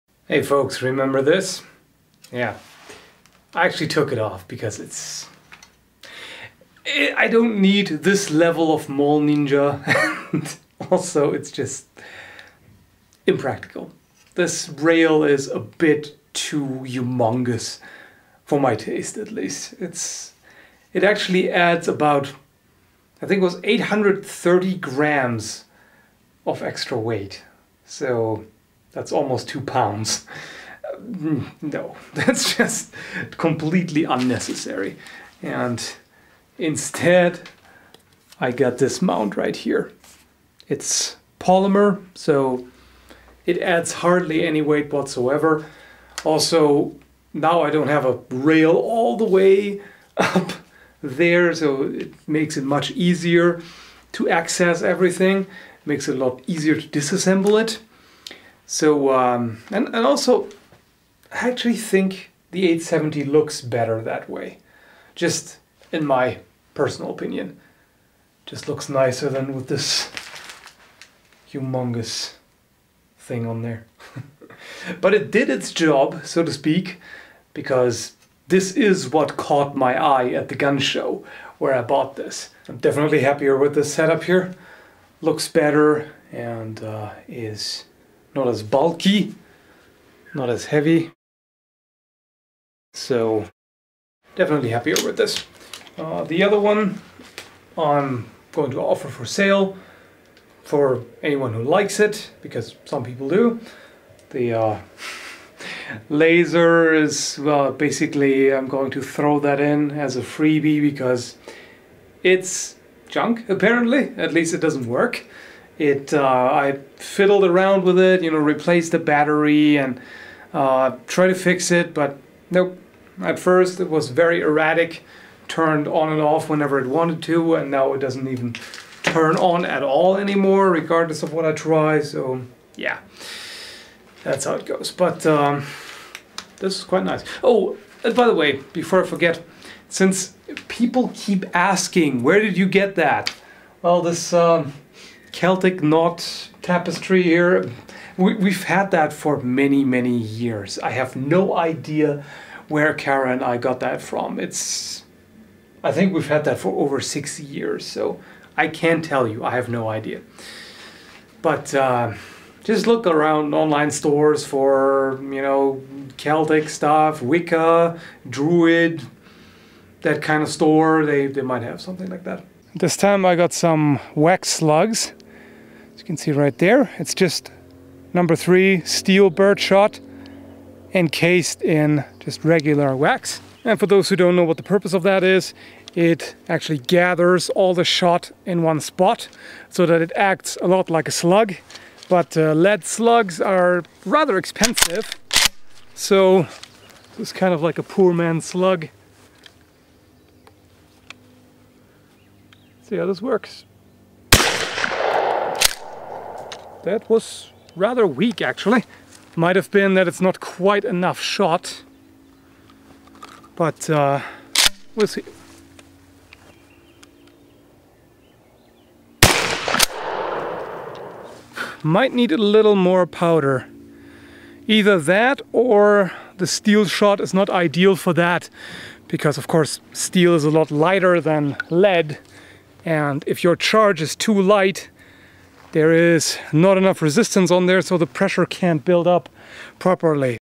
The new look of my Remington 870 Express Tactical
Got a polymer mount now instead of the heavy, bulky quad-rail that was on there when I got it at the gun show. In my opinion it's actually nicer now.

Also tried to shoot two wax slugs but that didn't work too well. I guess I need to experiment some more to get them right. The first shell felt rather weak (possibly too little weight to build up enough pressure), and apparently in both case the wax broke up. The shot still patterned closely but it didn't hit as one solid mass.